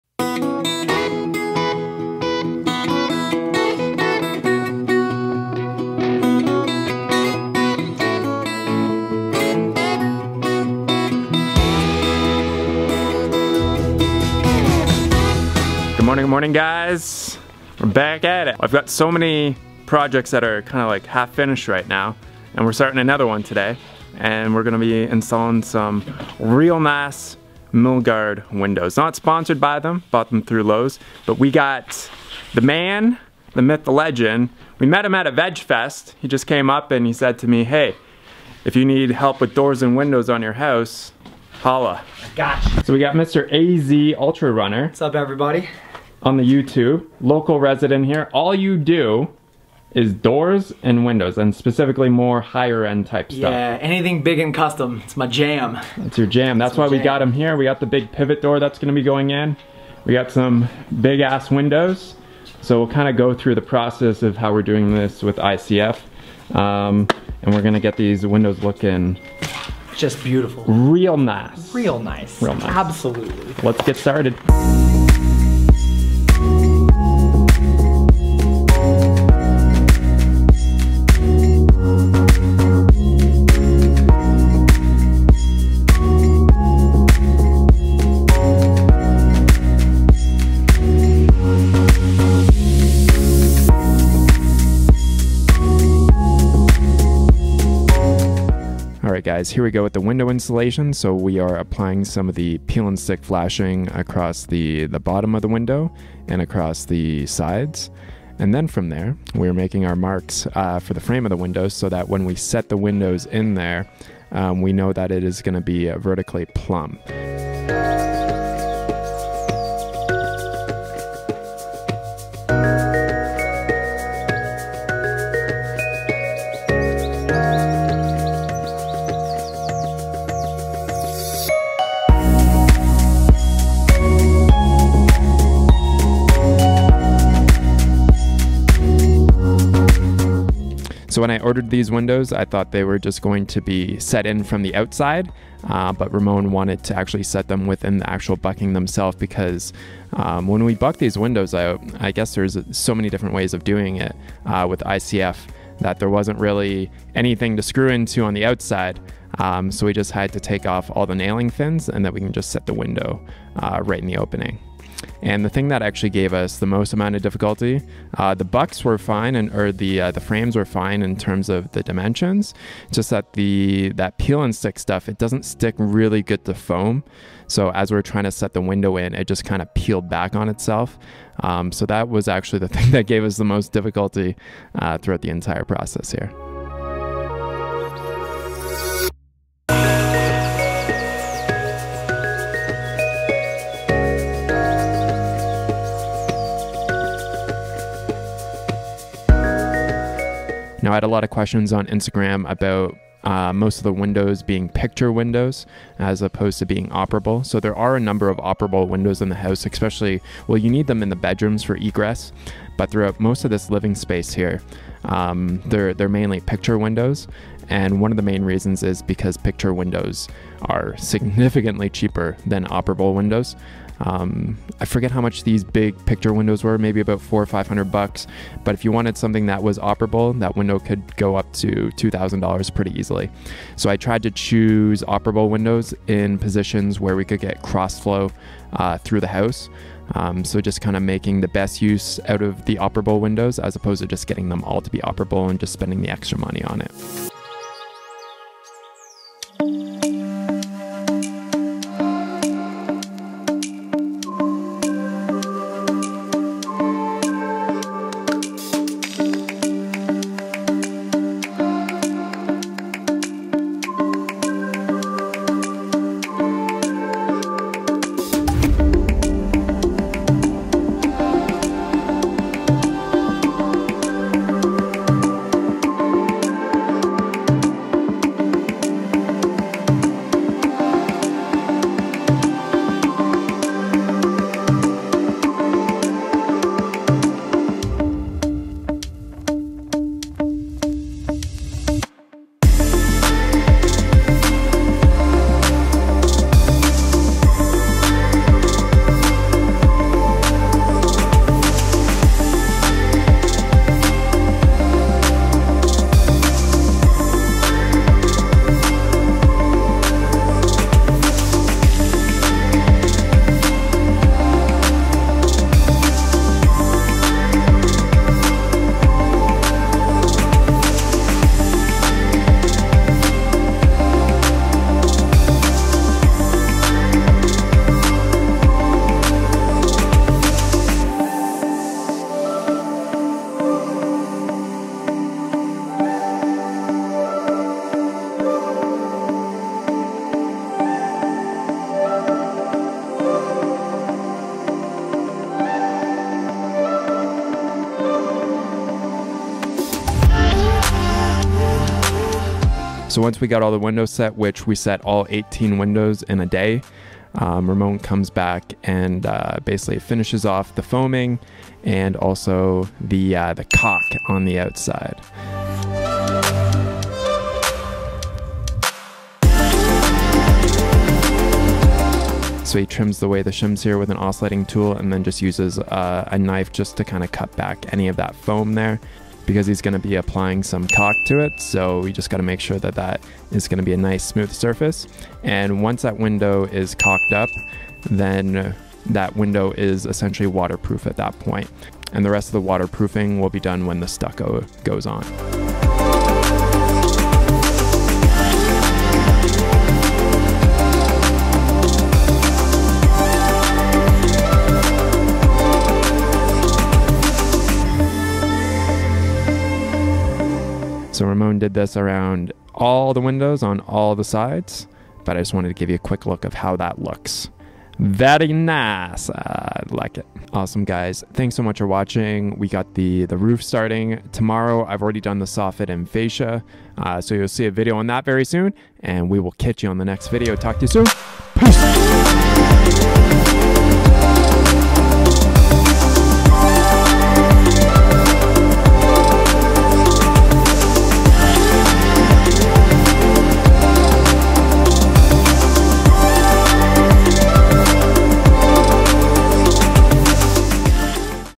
WHOA! Installed 18 Windows In A Day - Time lapse
📸 Follow the build on Instagram